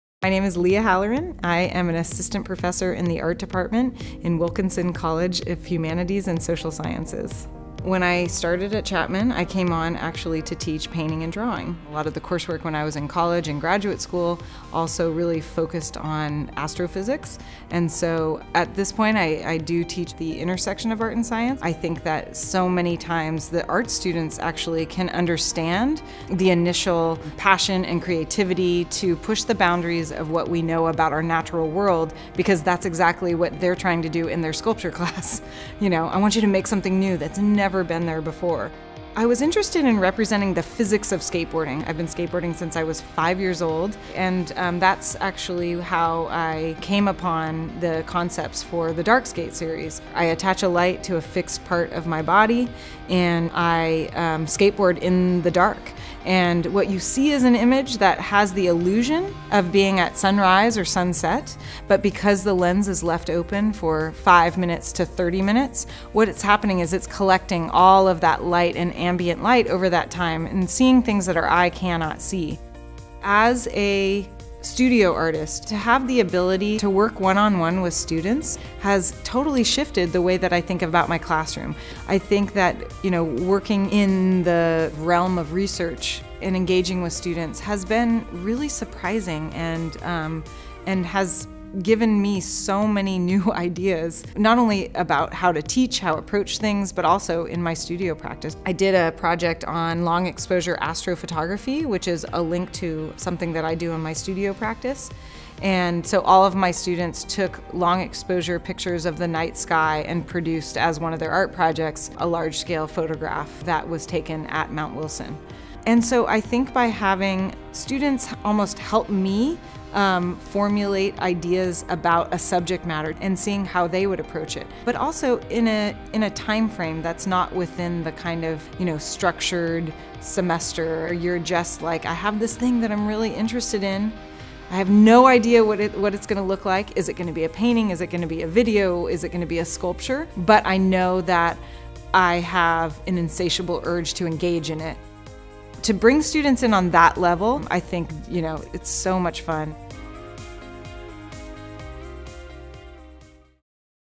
Lia Halloran - The Intersection of Art & Science
Skateboarding, astronomy, Jet Propulsion Labs, Chapman University Professor Lia Halloran uses all of these to create unique art and teach her students that art is all around us. With a personal interest in astrophysics, Halloran challenges her students to push the boundaries when thinking of art just as scientists push the boundaries in their field. Discover more about the arts at Chapman University